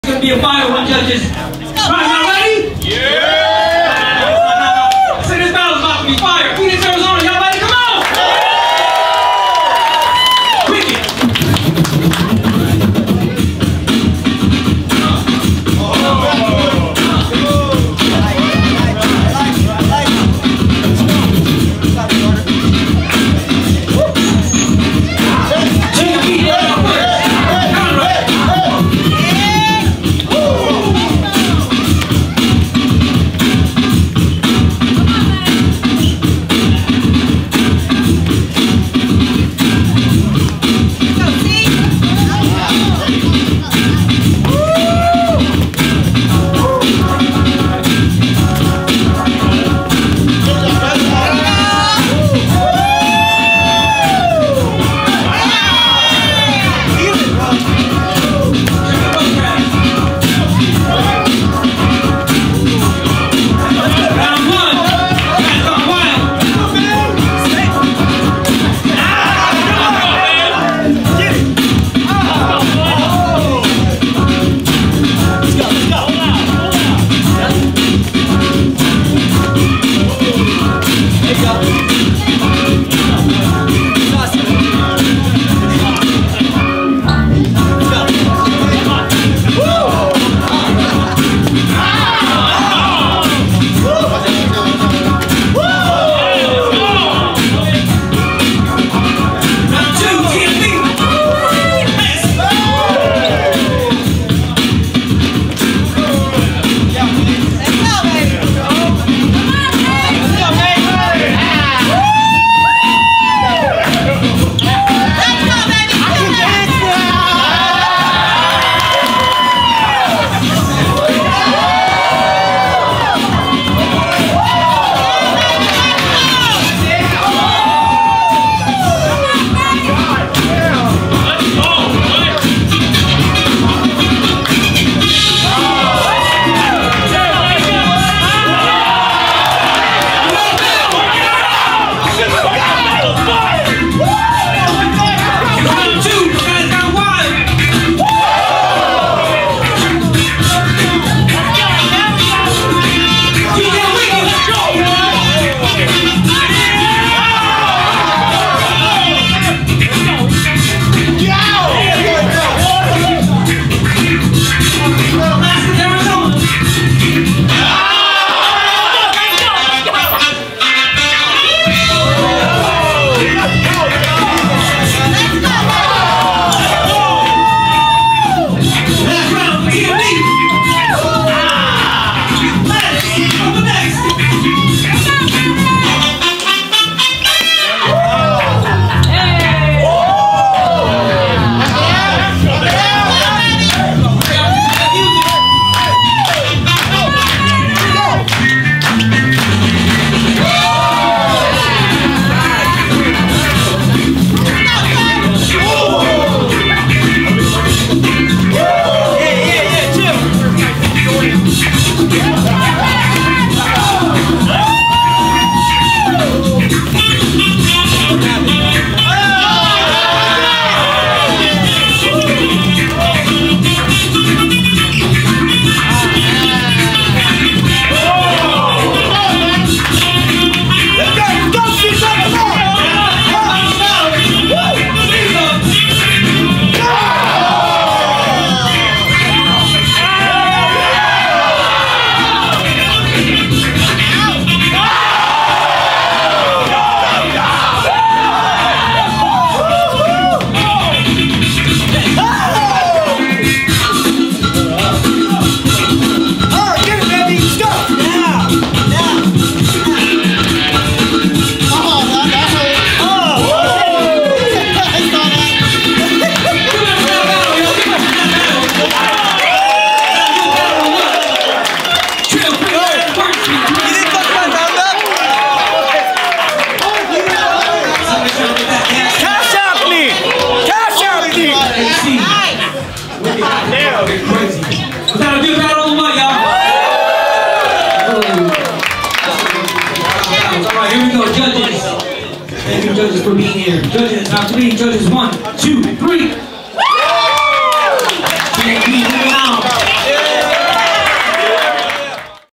GUYS GONE WILD VS TMB at TRILL 3 Year Anniversary
WINNER: T.M.B
JUDGES: FLO MASTER, KEN SWIFT & VENUM
HOSTED BY BBOY HOUSE
PHOENIX,AZ